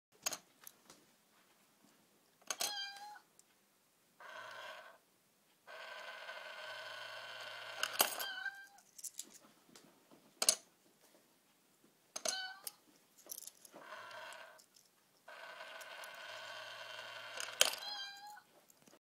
Novelty money box
Look what crap Erin conned me into buying her at meadowhell